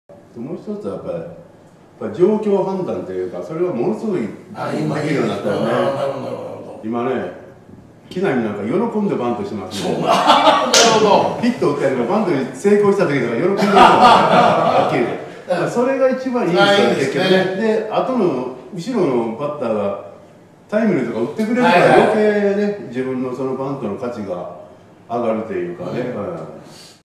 【岡田監督】木浪は喜んでバントする。…フル動画はコメント欄URLから！
【掛布×岡田】シーズン中に豪華対談が実現！今の阪神のすべてが分かる！交流戦の戦い方や岡田采配の裏側まで…！